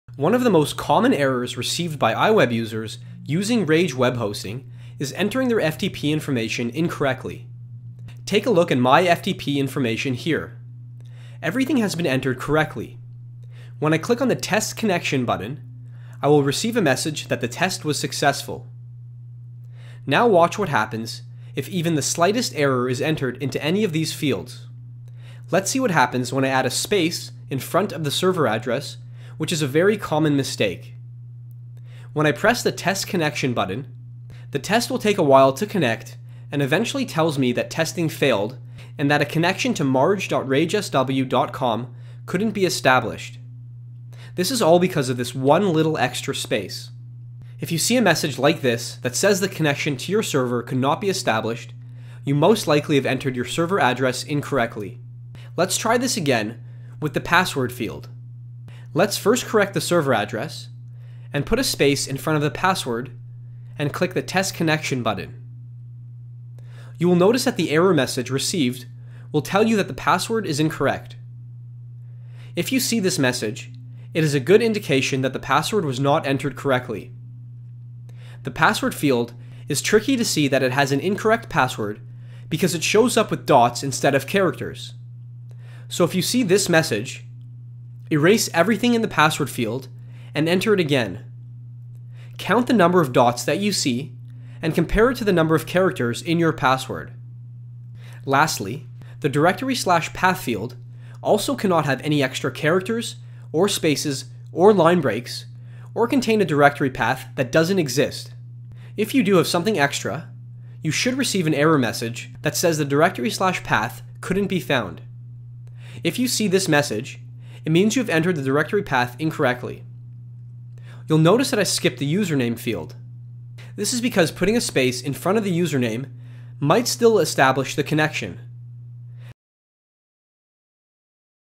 Publishing From iWeb To RAGE Web Hosting - Troubleshooting
Troubleshooting tips on publishing from iWeb to your RAGE Web Hosting account.